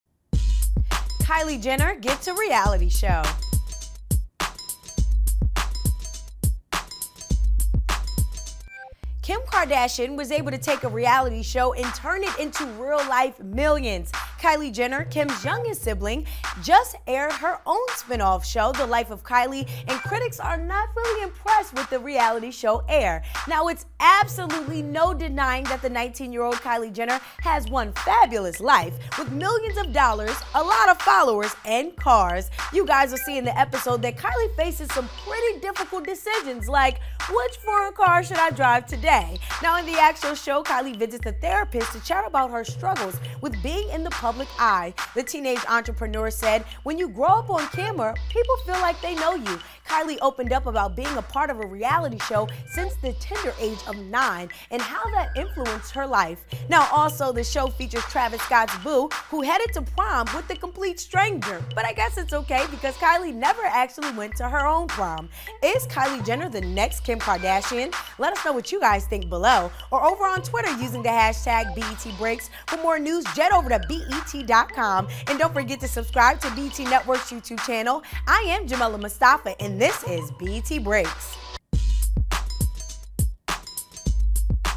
Kylie Jenner Gets A Reality Show - BET Breaks
Critics were not impressed with "The Life of Kylie."